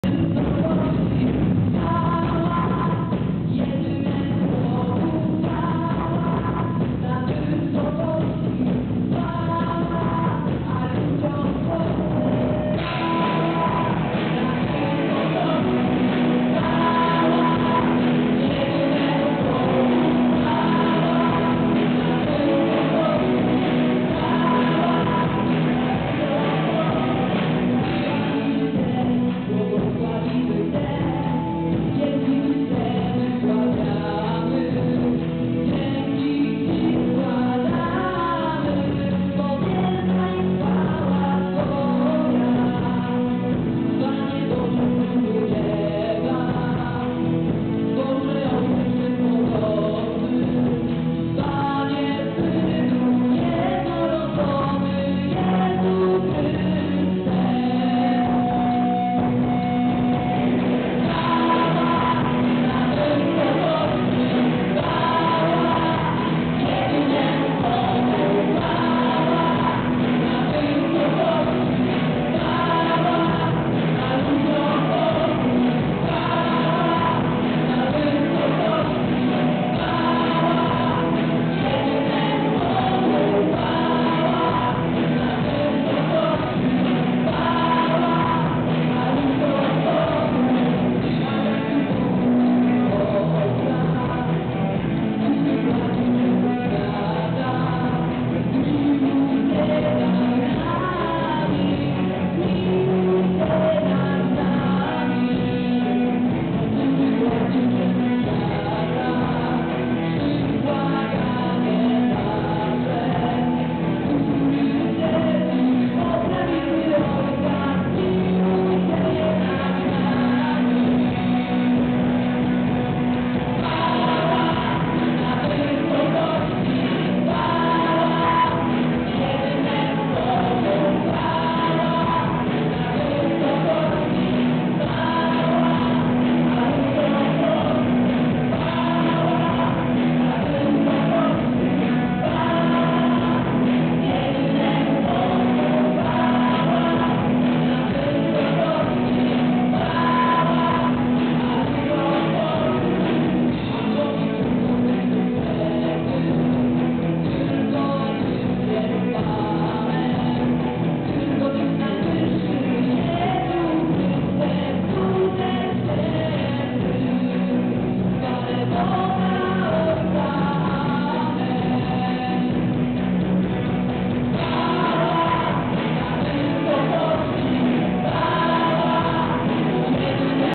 rockowa msza 1.MOV
Pierwsza w historii Świnoujścia rockowa msza święta